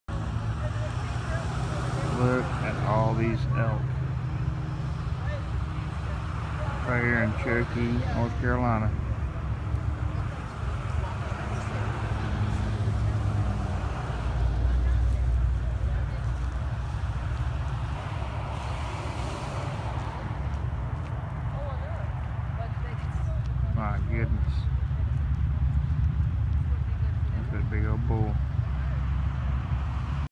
Elk Herd Cherokee, NC
Elk herd in the Great Smoky Mountains National Park Cherokee NC October 31 2015